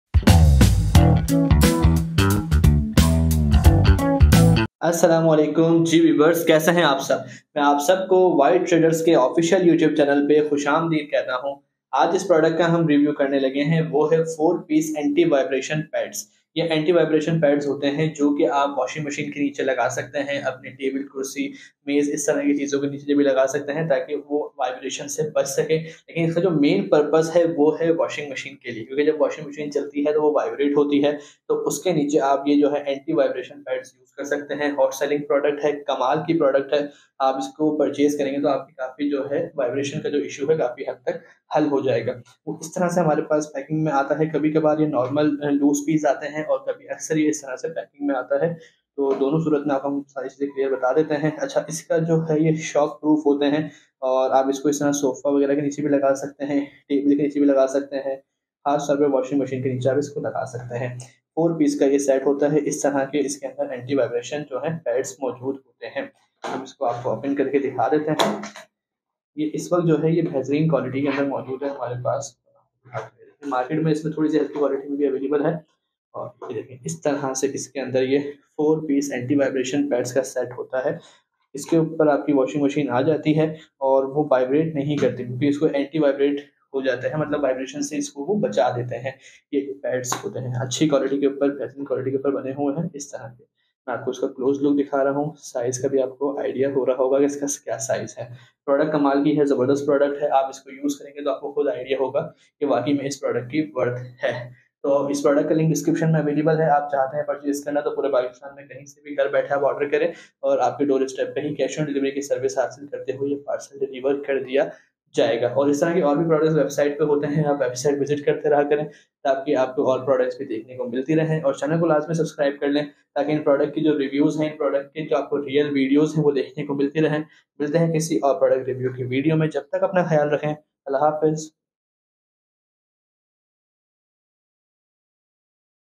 4PCS ANTI VIBRATION PADS FOR WASHING MACHINE | Unboxing & Review
In this video we will give you review about 4Pcs Anti Vibration Pads for Washing Machine.

🛒 WhatsApp and Catalogue Number: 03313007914